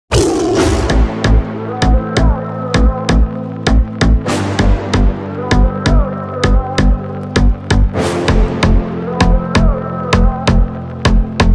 The St. Bernard & corgie play together puppy st. Bernard
So cute and funny